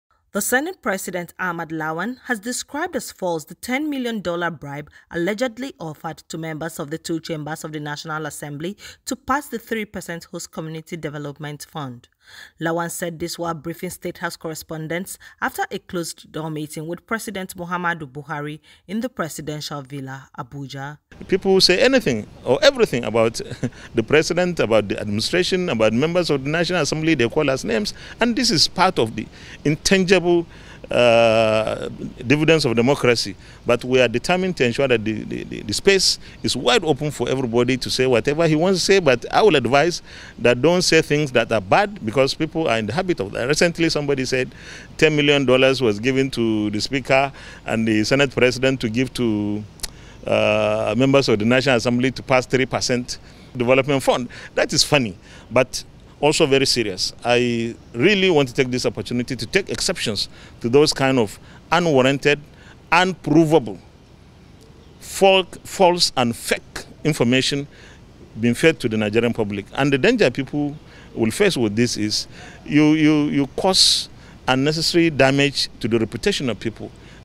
Senate President Lawan meets Buhari, dismisses allegation of $10m bribe
Senate President Ahmad Lawan has described as false the $10million bribe allegedly offered to members of the two Chambers of the National Assembly to pass 3% host community development fund.